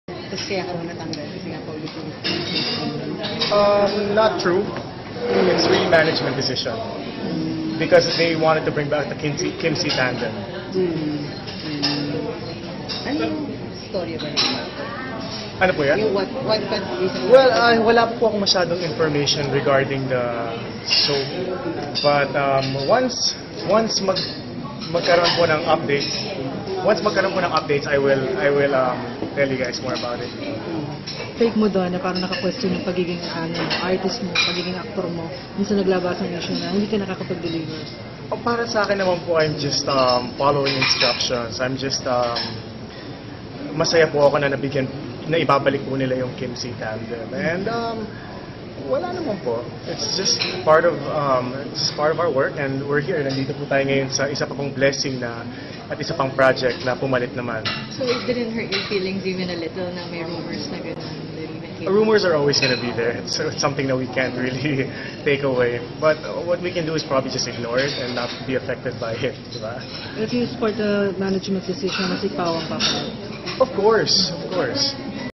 Xian Lim Explains Why He Is No Longer Part Of The Upcoming Series Bridges
Xian Lim denied rumors that he was removed from the cast of the upcoming series "Bridges" because he could not keep up with the performances of lead stars Maja Salvador and Jericho Rosales.